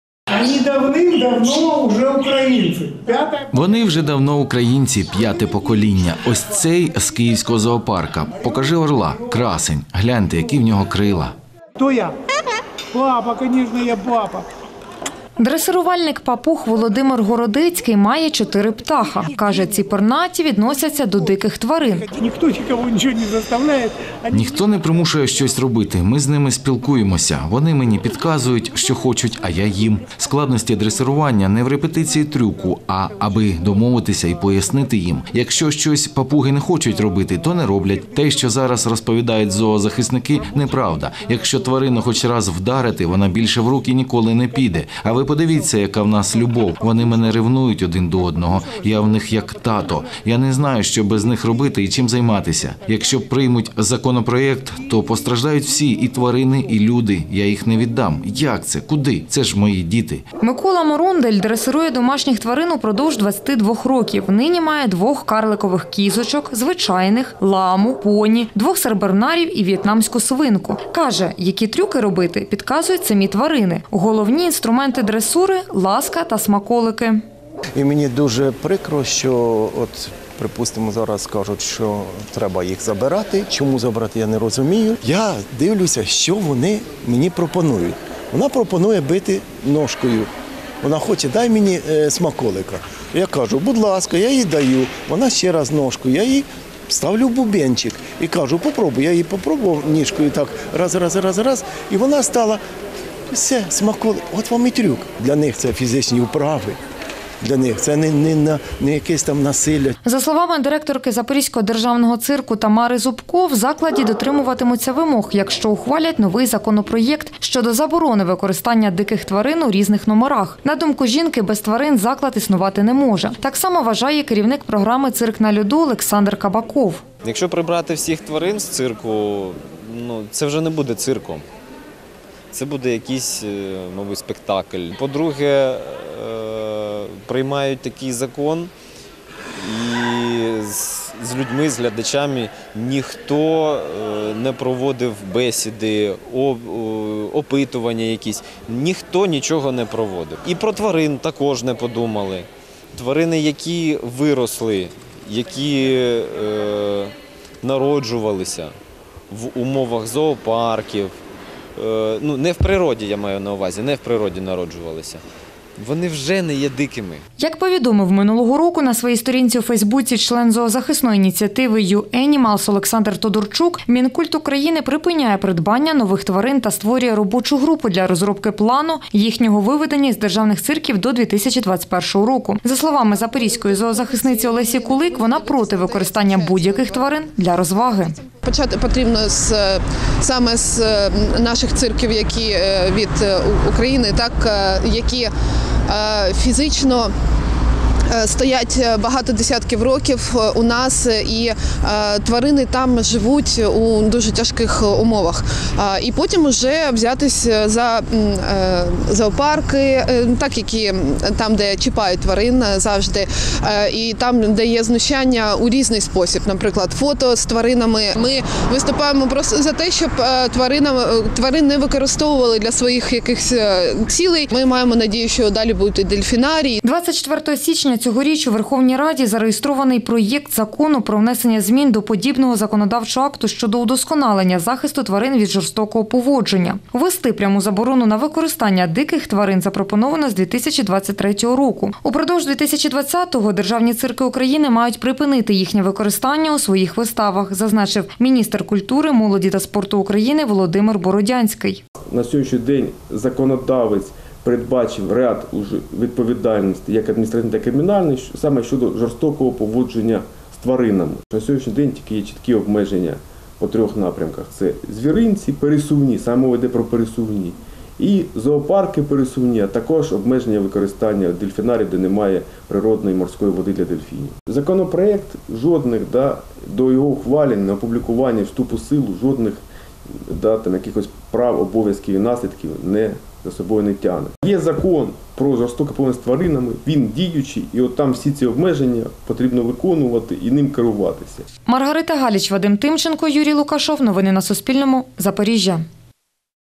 Новини - Цирк без тварин - 03.02.2020
Циркові вистави з дикими та свійськими тваринами поки ще можна побачити в Україні. З приватними шапіто співпрацюють державні цирки, що підпорядковані Міністерству культури. Зокрема, це сім стаціонарних закладів у Києві, Одесі, Харкові, Дніпрі, Запоріжжі, Кривому Розі та Львові та ще п’ять на окупованих територіях. Цими днями в державному стаціонарному Запорізькому цирку приймають дресирувальників з їхніми пернатими та чотирилапими артистами. Нині власники тварин та керівники закладів занепокоєні, як працюватимуть далі - після прийняття законопроекту щодо заборони використання диких тварин та тих, які важко піддаються дресируванню. Подробиці – далі у сюжеті.